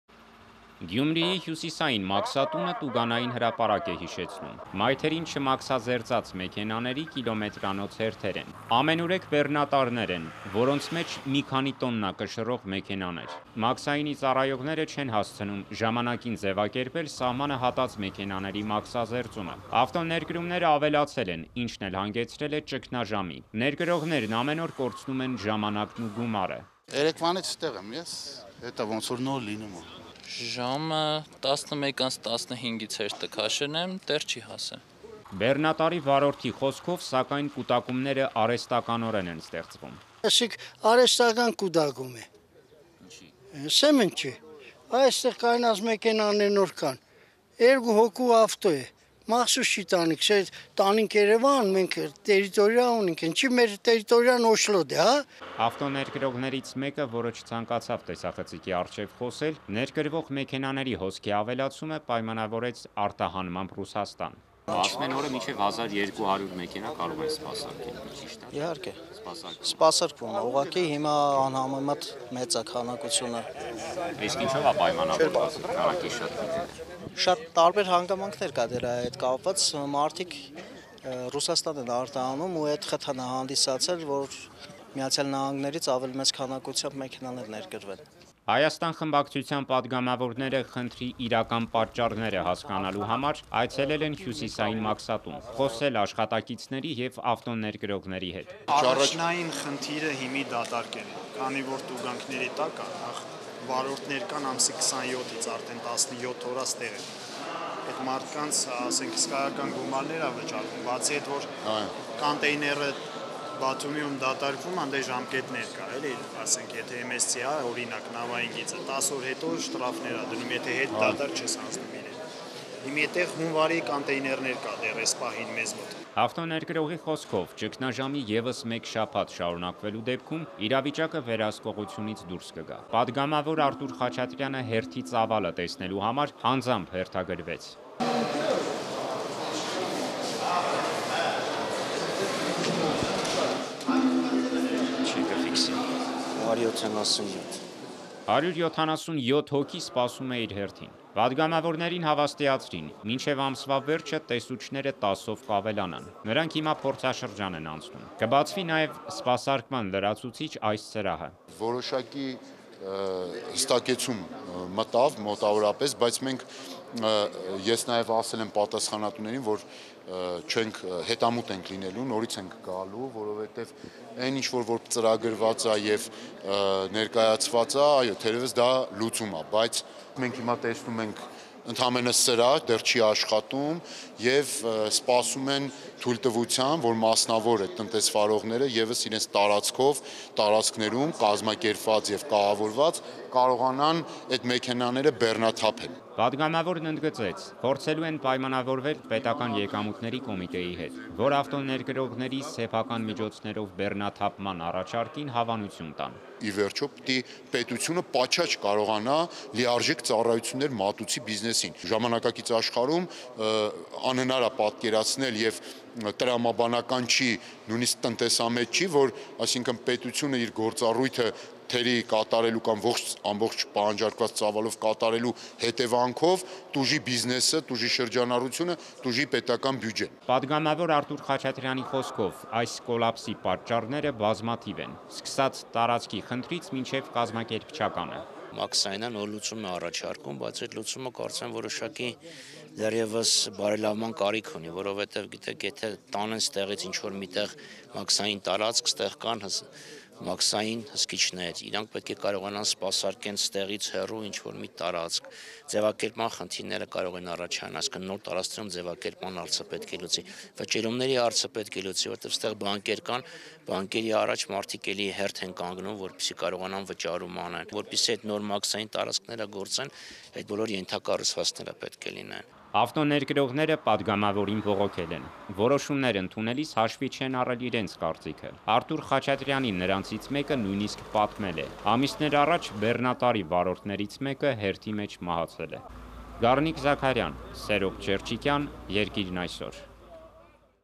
Չմաքսազերծած մեքենաների կիլոմետրանոց հերթեր` Գյումրիում
Մանրամասները՝ Գառնիկ Զաքարյանի տեսանյութում